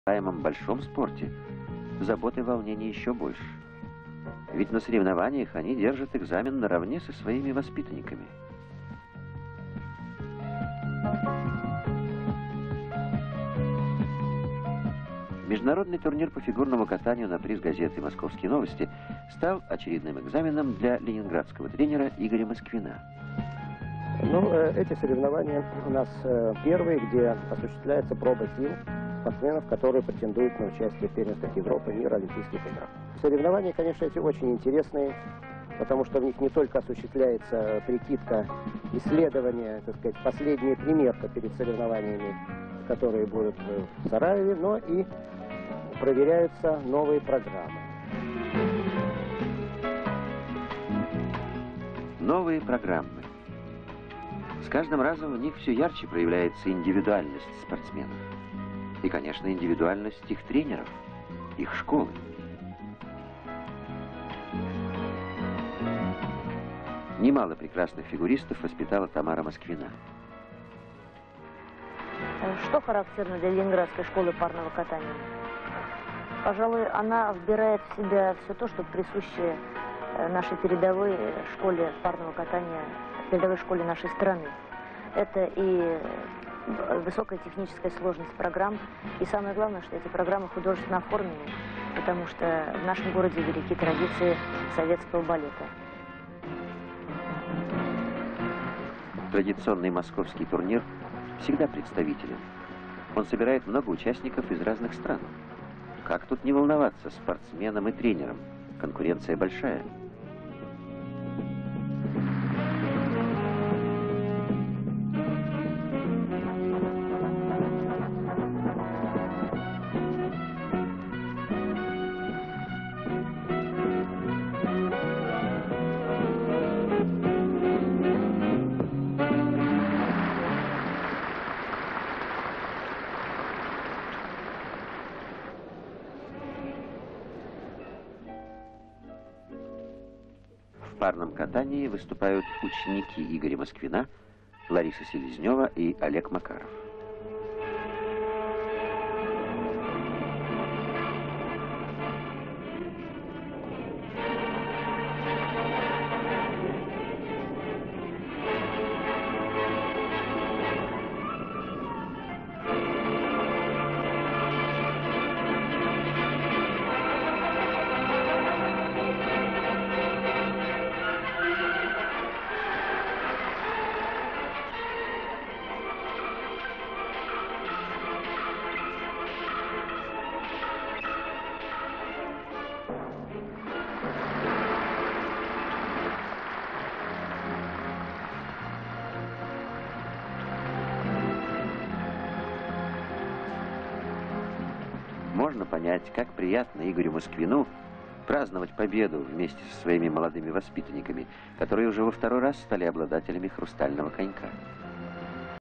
FIGURE SKATING COACH I. MOSKVIN; SOVIET UNION GYM TRAINING, INTERVIEW WITH COACH T. MOSKVINA 1983
Figure skating coach Igor Moskvin talks with skaters O. Makarov and L. Selezneva during a training session. Russian figure skating teacher Irina Rodnina observes skaters training. Irina Moskvina commenting on the upcoming USSR competitions for the prize of the newspaper Moscow News. Skaters during a sports competition. Looks I. Moskvin. Skaters in training. Interviews figure skating coach Tamara Moskvina (synchronously). STOCK FOOTAGE Fragments of skaters ’appearances at international games for the prize of the Moscow News newspaper - single and double skating.
Digitised in HD and SD from Soviet film / Newsreel, circa 1983 - featuring other Soviet Sport